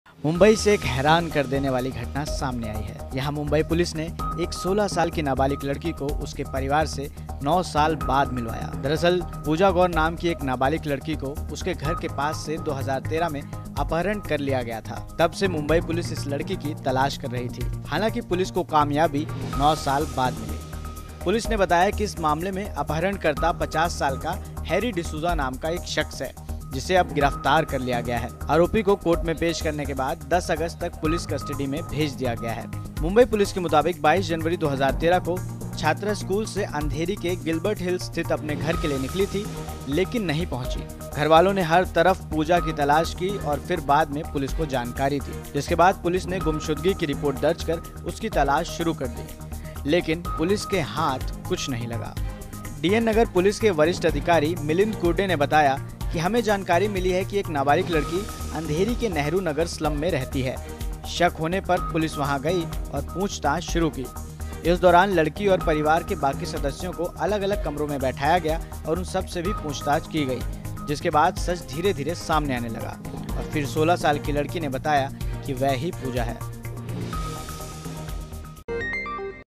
Crime News: 9 साल बाद मुंबई पुलिस ने अगवा मासूम को खोज निकाला, स्कूल से लौटते वक्त हुआ था अपहरण
स्कूल से लौट रही मासूम को मुंबई (Mumbai) में अगवा कर एक शख्स ने घर में नौकरानी बना कर रखा था. जिसे मुंबई पुलिस ने 9 साल बाद परिवार से मिलवाया. नाबालिग अब 16 साल की हो चुकी है.